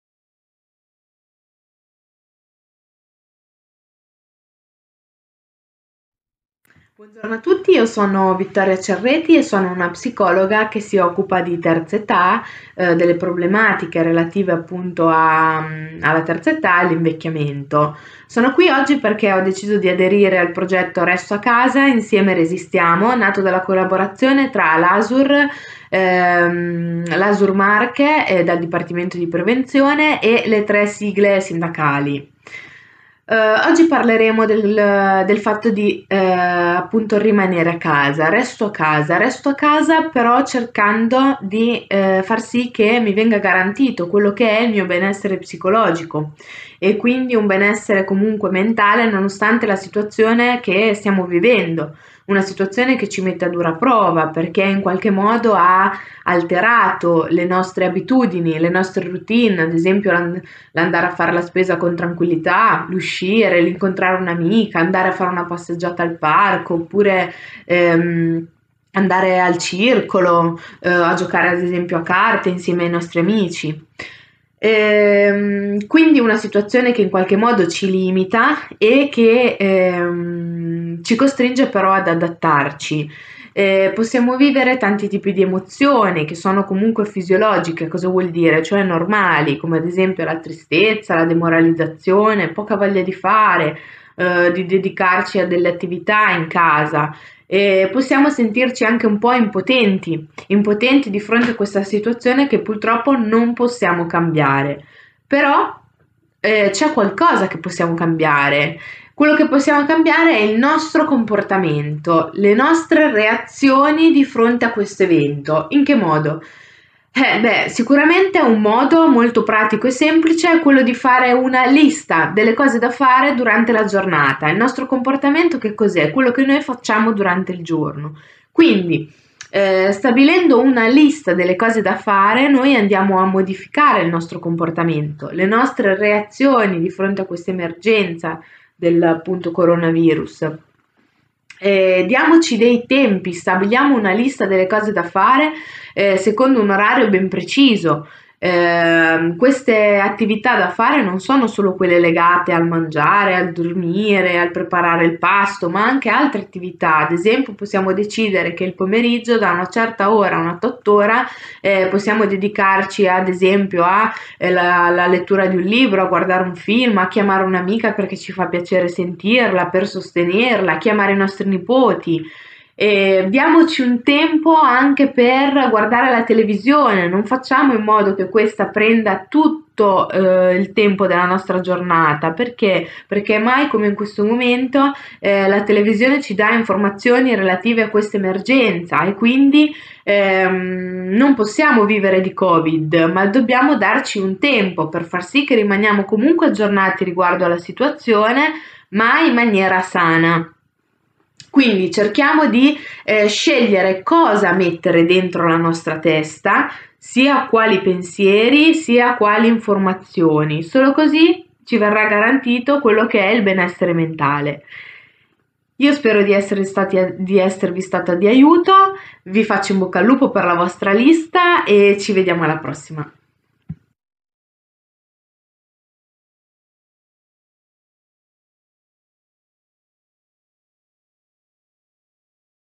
Io resto a casa, insieme resistiamo. Video n. 1 della Dottoressa Vittoria Cerreti
La Dott.ssa Vittoria Cerreti, Psicologa esperta in psicologia dell'invecchiamento, con alcune indicazioni utili per restare a casa in questi giorni di emergenza sanitaria